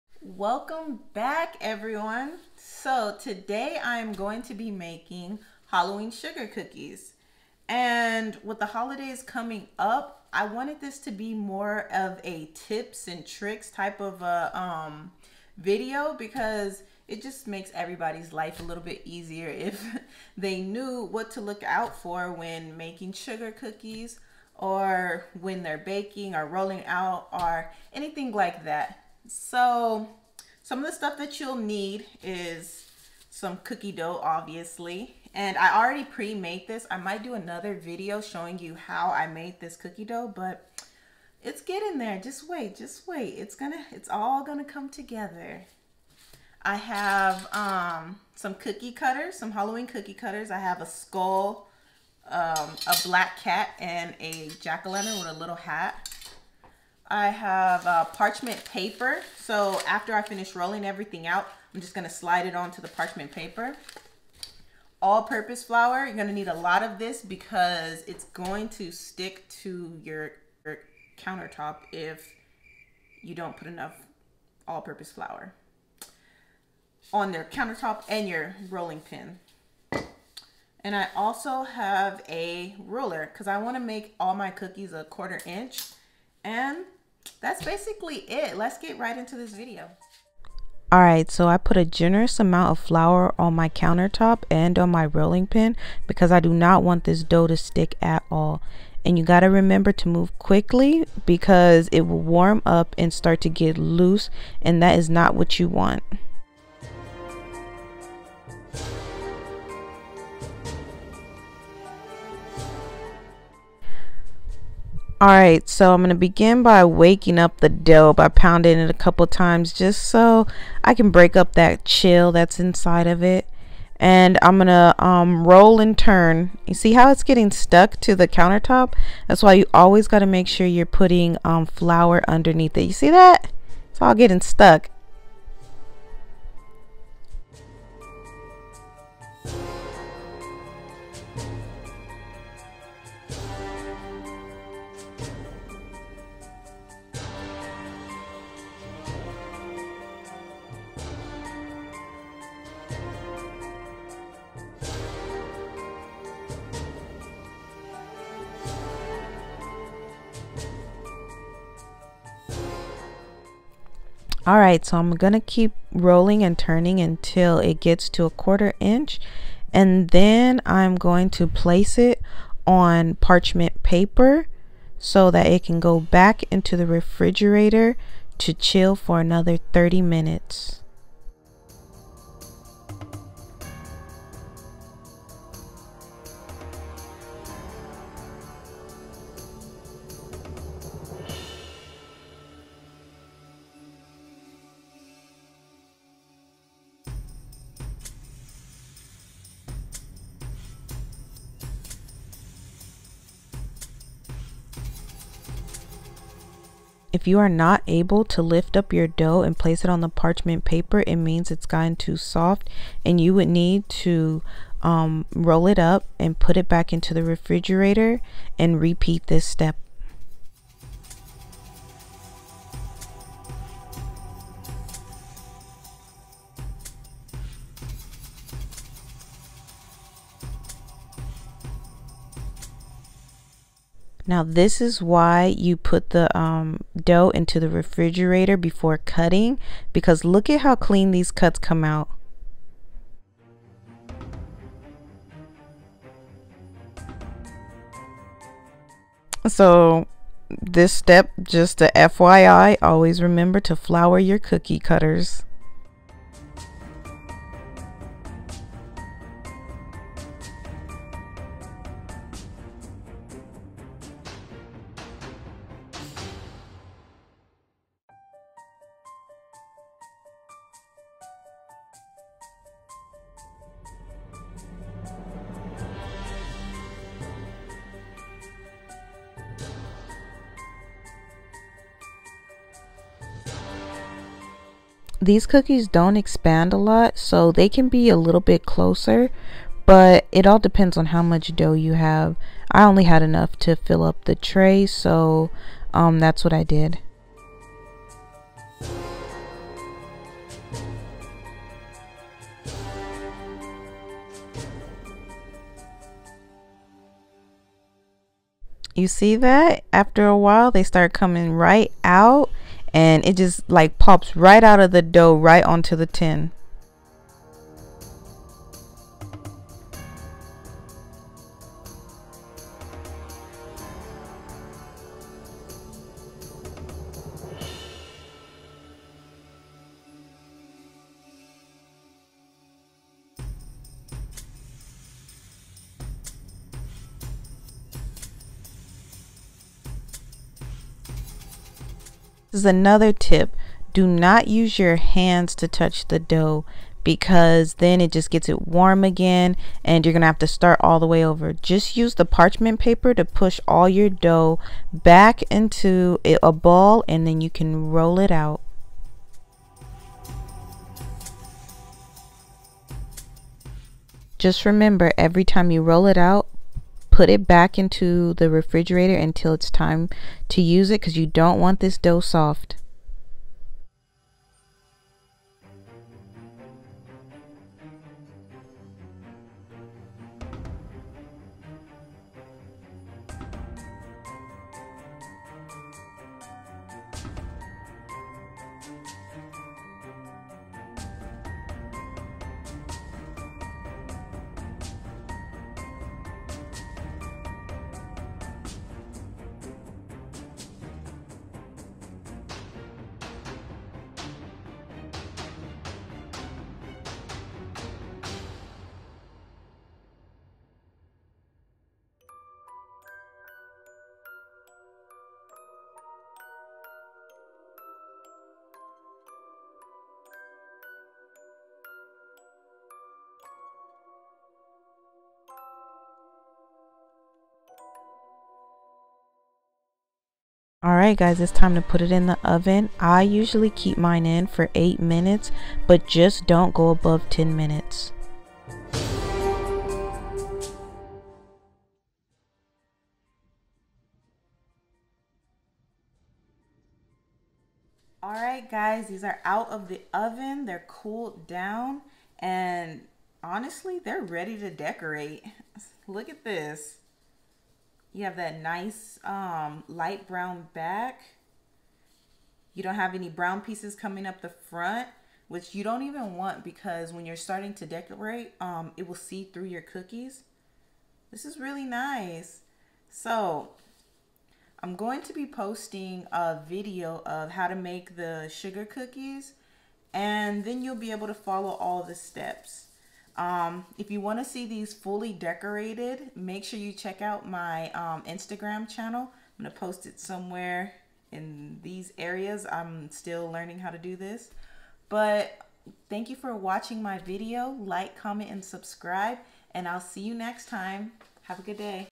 Halloween Cookies | Tips And Tricks To Cut Out A Perfect Sugar Cookie | LB Sweetshop | Bakery
Hello everyone! So this week is all about sugar cookies. How to get perfect cut outs for the holidays. No cutting after you bake.